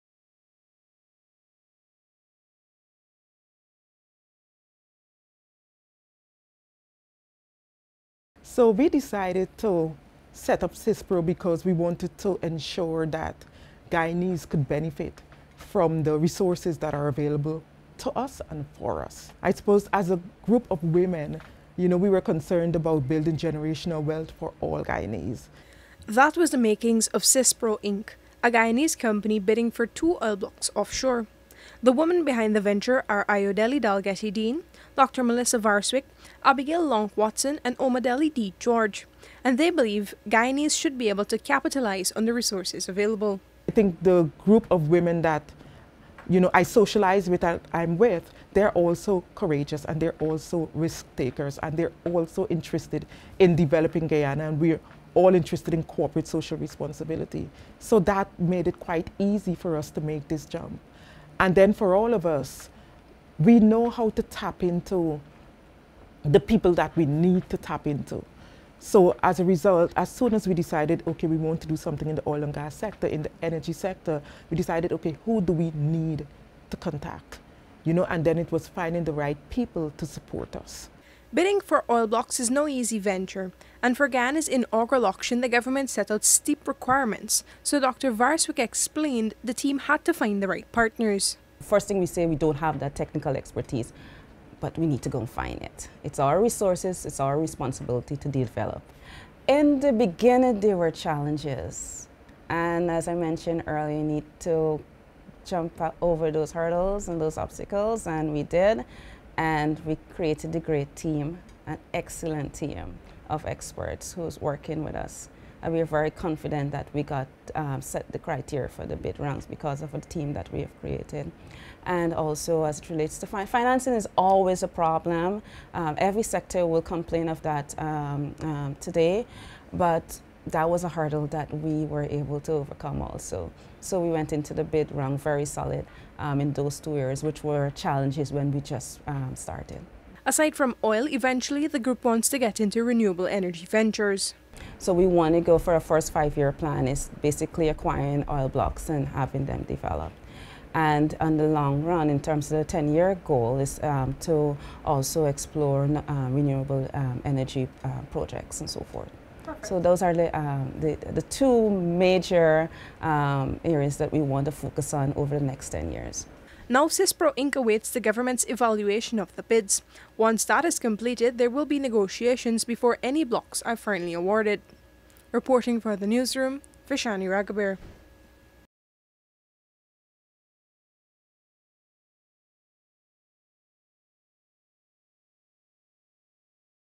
FROM OIL BLOCKS TO RENEWABLE ENERGY, FOUR GUYANESE WOMEN SET ON MAKING THEIR MARK
Guyana’s prolific oil and gas sector is expected to expand this year with the upcoming award of new oil blocks following the country’s inaugural auction and four Guyanese women, who founded Sispro Inc., want to be part of that expansion. Two of the four founding directors, Dr. Melissa Varsyck and Ayo Dalgety- Dean, told the News Room more about the company and its plans for Guyana. Vishani Ragobeer reports…